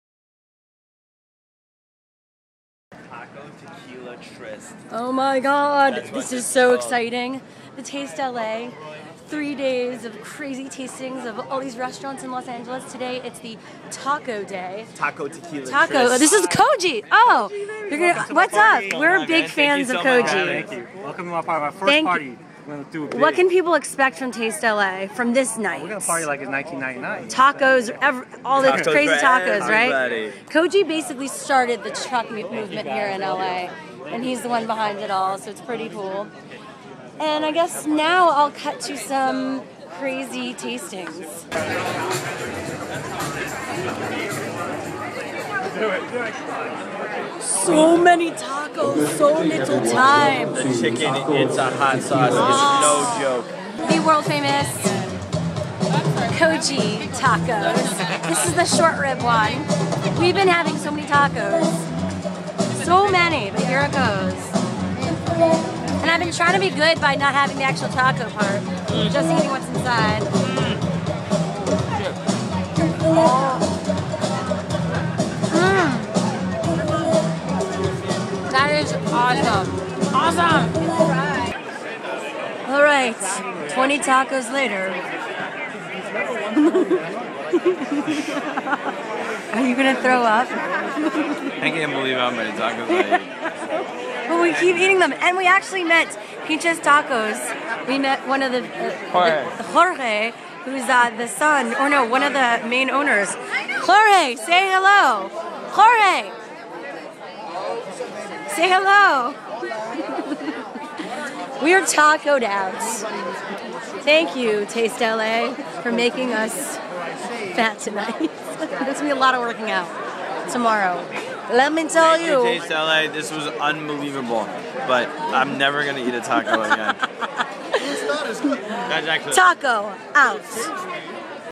Vlog: Taco Madness
Shira Lazar heads to Taste LA Taco night and indulges in some of the best tacos in the city